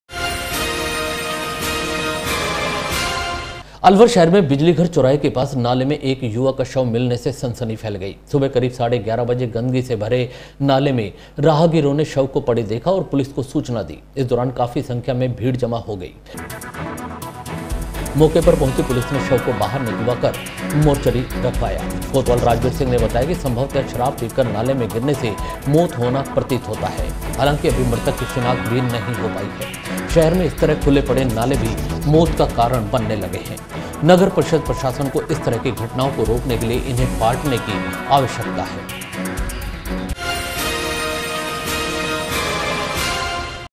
बिजली घर चौराहे के पास नाले में मिला शव, पुलिस पहुंची मौके पर,
अलवर शहर में बिजली घर चौराहे के पास नाले में एक युवक का शव मिलने से सनसनी फैल गई। सुबह करीब साढ़े ग्यारह बजे गंदगी से भरे नाले में राहगीरों ने शव को पड़े देखा और पुलिस को सूचना दी। इस दौरान काफी संख्या में भीड़ जमा हो गई। मौके पर पहुंची पुलिस ने शव को बाहर निकलवा कर मोर्चरी रखवाया। कोतवाल राजवीर सिंह ने बताया कि समंभवतया शराब पीकर नाले में गिरने से मौत होना प्रतीत होता है। हालांकि अभी मृतक की शिनाख्त भी नहीं हो पाई है। शहर में इस तरह के खुले पड़े नाले भी मौत का कारण बनने लगे है। नगर परिषद प्रशासन को इस तरह की घटनाओं को रोकने के लिए इन्हें पाटने की आवश्यकता है।